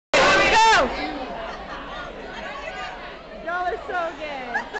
Walk it out!
Me and my homeboy Darryl D decided to walk the red carpet at homecoming. (Yes, we know we fly!)